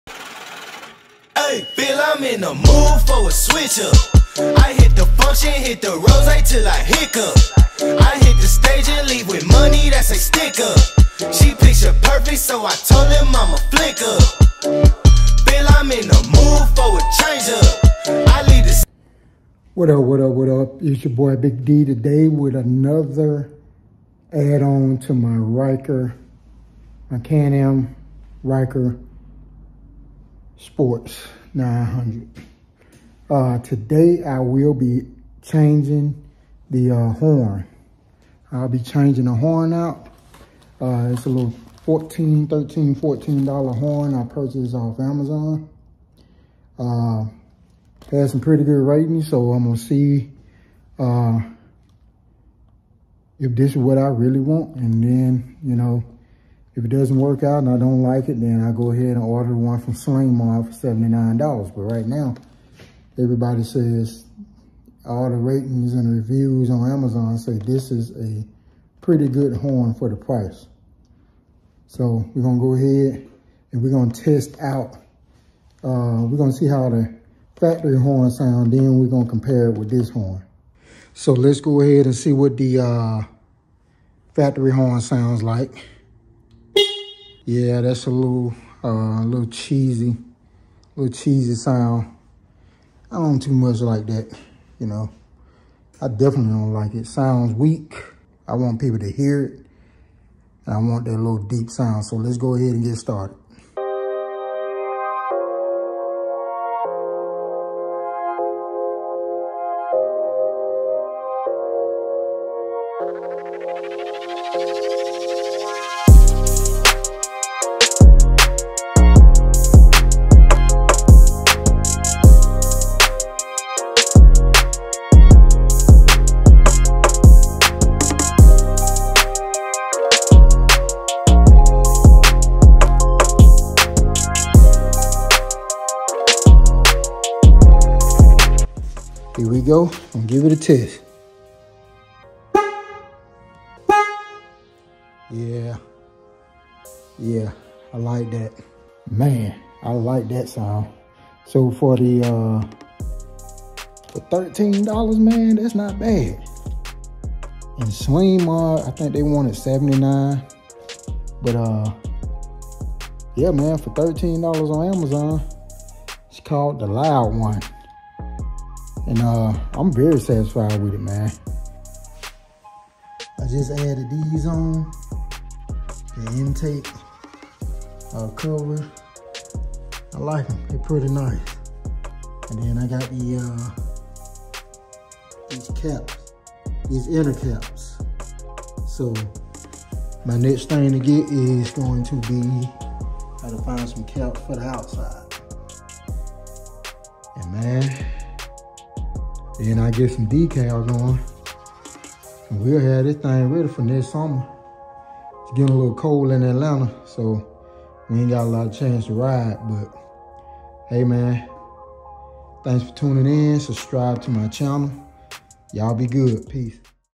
Can Am Ryker $13 Horn Upgrade
Upgraded my horn on my Can Am Ryker for $12.70 off Amazon.
Sounds better and easy to install.